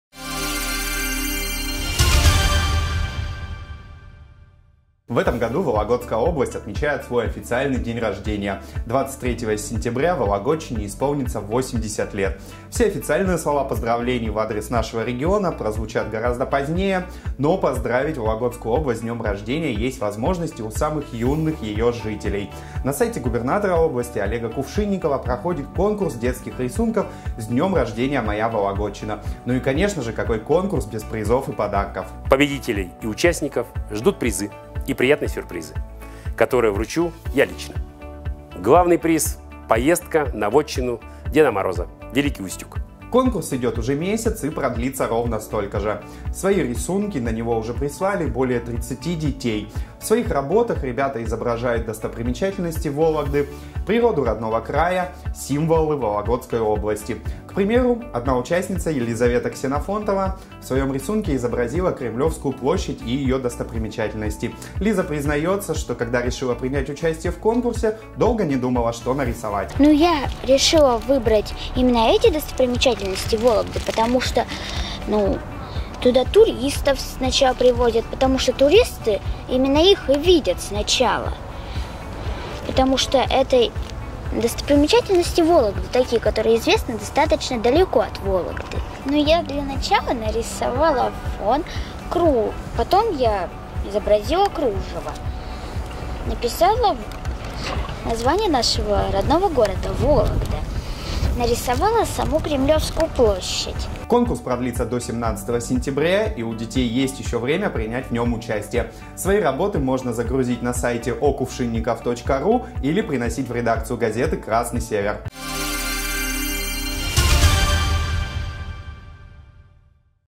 Продолжается конкурс детских рисунков посвященных юбилею Вологодской области
В этом году Вологодская область отмечает свой день рождения. 23 сентября ей исполняется 80 лет. Все официальные слова в адрес нашего региона прозвучат позднее, но поздравить родную область с юбилеем могут и ее юные жители. Сделать это можно в творческой форме. На сайте губернатора области Олега Кувшинникова объявлен конкурс детских рисунков «С днем рождения, Вологодчина!». Но какой конкурс без призов?
Конкурс идет уже месяц и продлиться еще столько же. На него уже заявились более 30 участников. В своих работах дети изображают достопримечательности Вологодчины, природу родного края, символы Вологодской области. К примеру, участница Елизавета Ксенофонтова представила на конкурс рисунок, на котором изображена Кремлевская площадь. Лиза рассказывает, что когда решила принять участие то долго не думала, что нарисовать.
Конкурс продлится до 17 сентября и у детей еще есть время принять участие. Свои работы можно загрузить на сайте губернатора okuvshinnikov.ru или принести в редакцию газеты «Красный север».